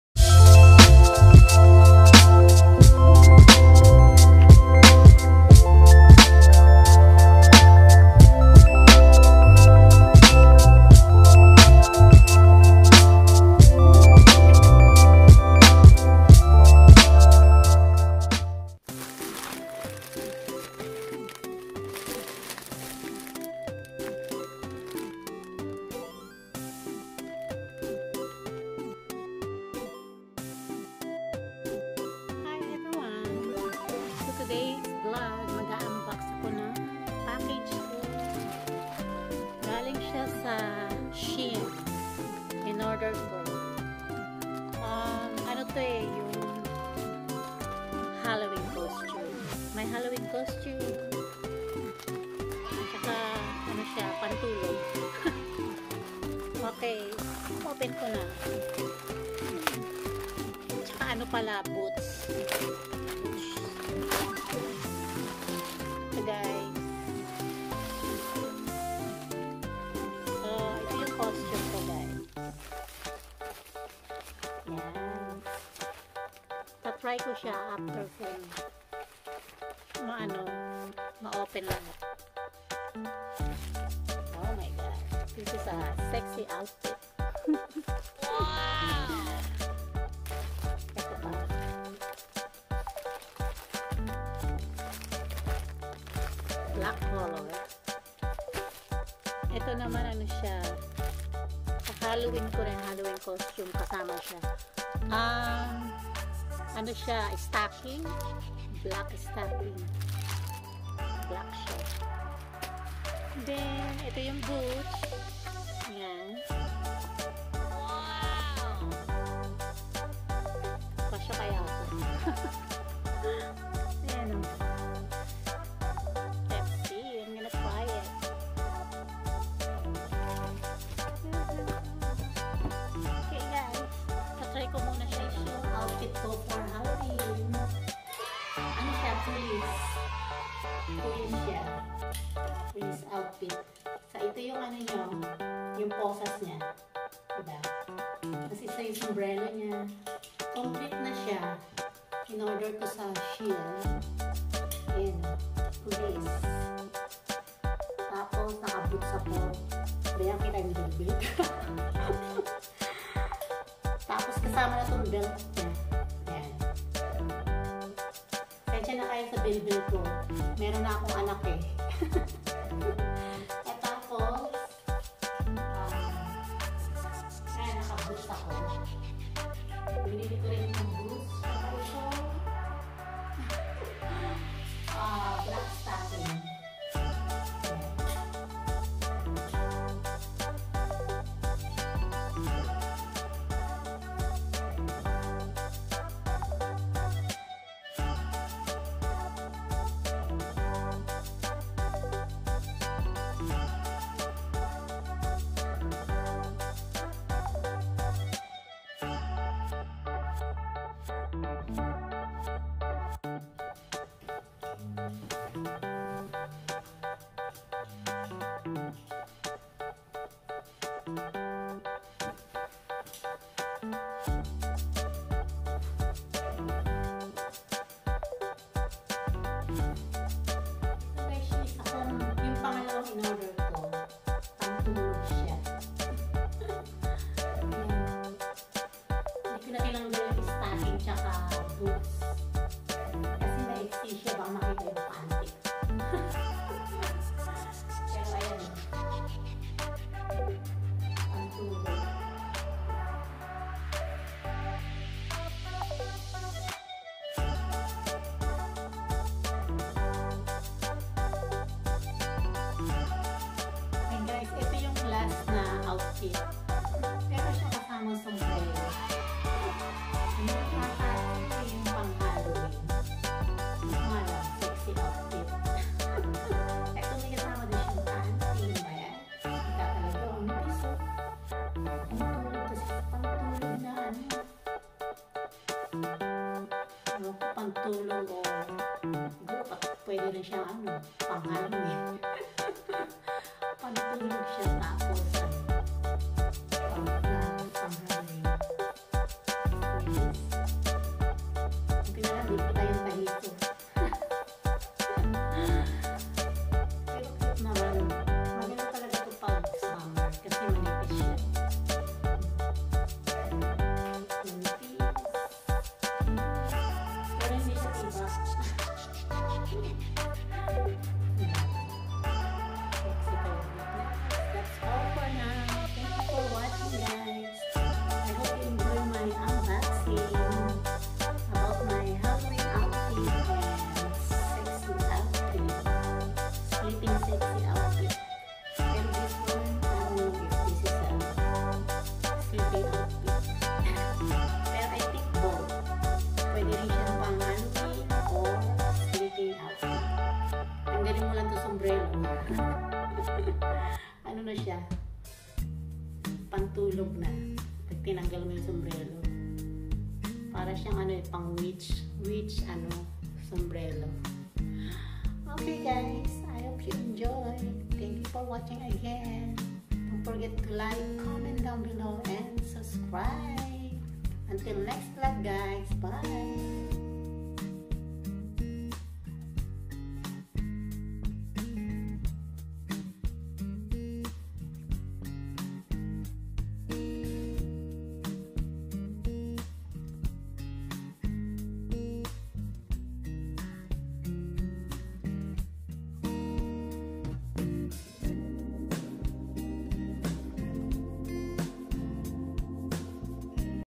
UNBOXING/ SHEIN HAUL/ HALLOWEEN COSTUME/SEXY SLEEPWEAR AND TRY ON
I love my Halloween costume and sleep wear not bad hehehe, thank you guys for Watching